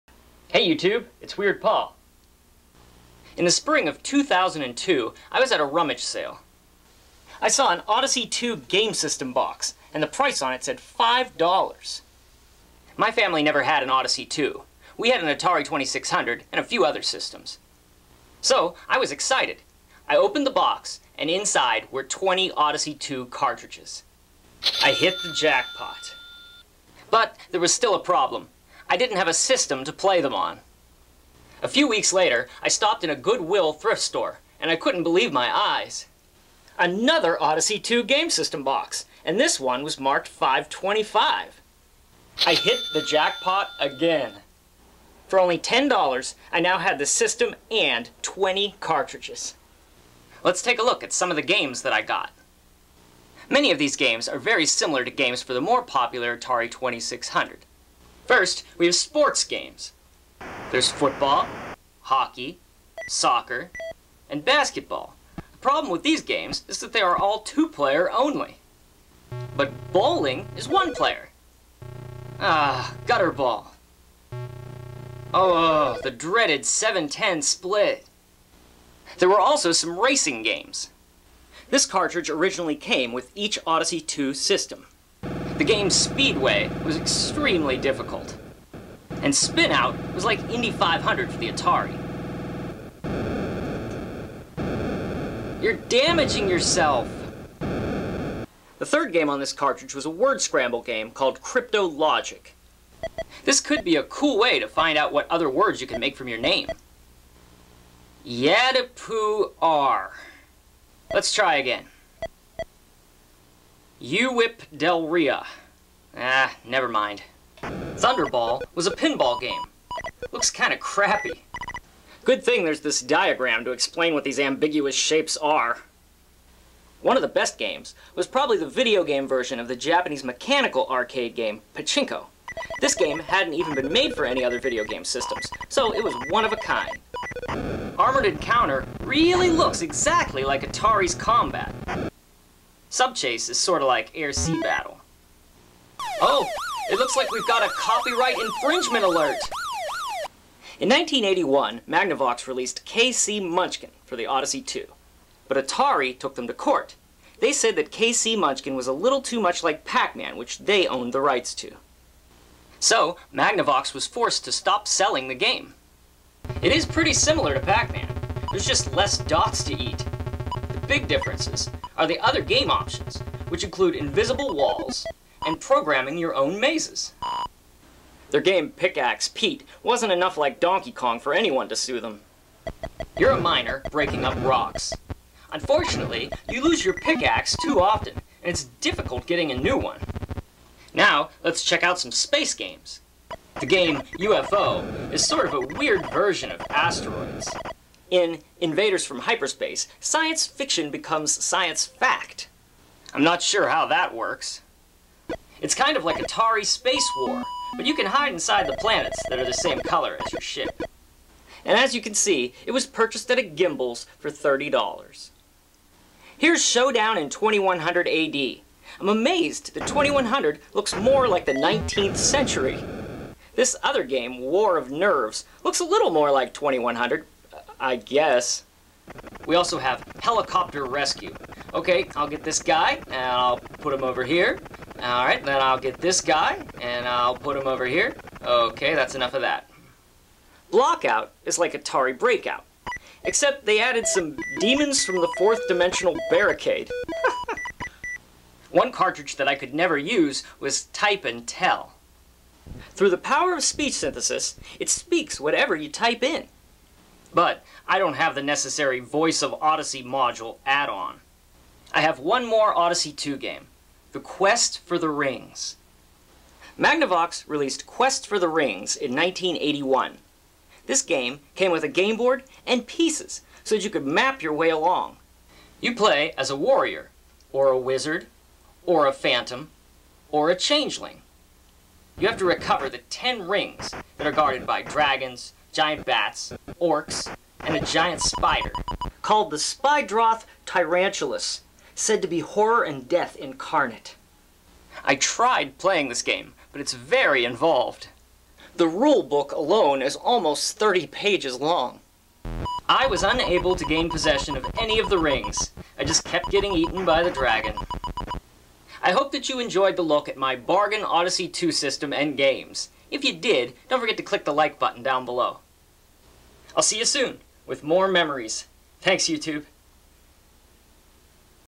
Odyssey 2 Video Game Console -(Weird Paul) Magnavox System Review II 80s
A look at my personal odyssey with the Magnavox ODYSSEY 2!

No copyright is claimed in the short clips used in this video.